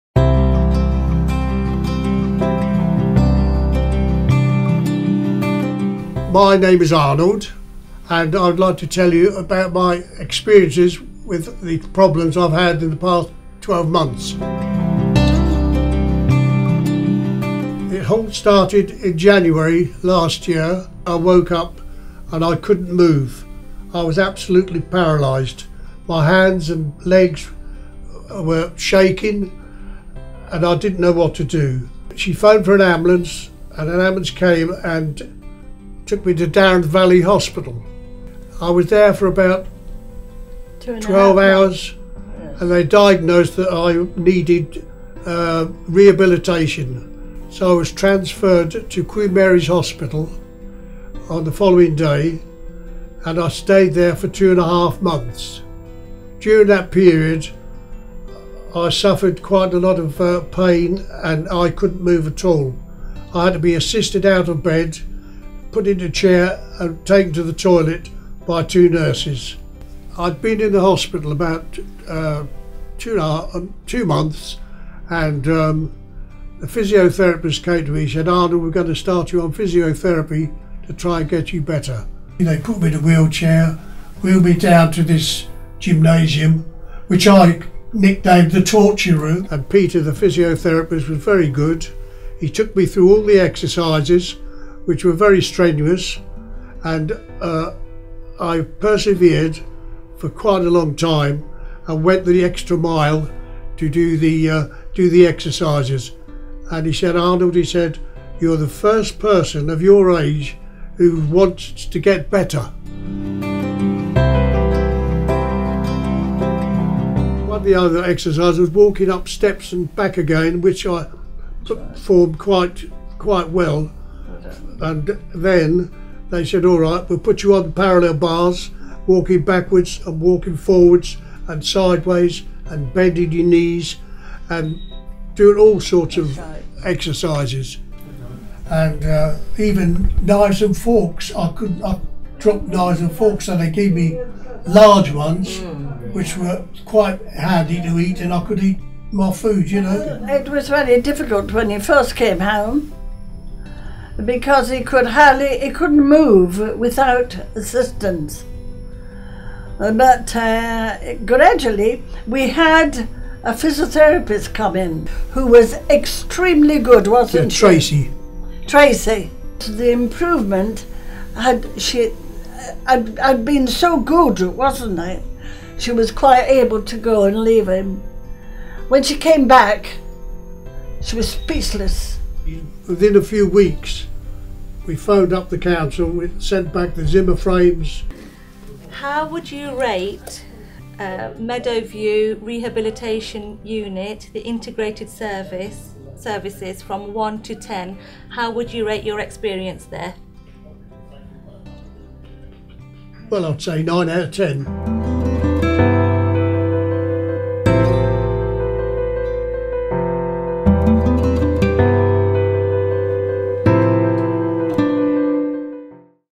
Meadow View
Dream Media were asked by Bexley Council to make a film about Stroke recovery and rehabilitation.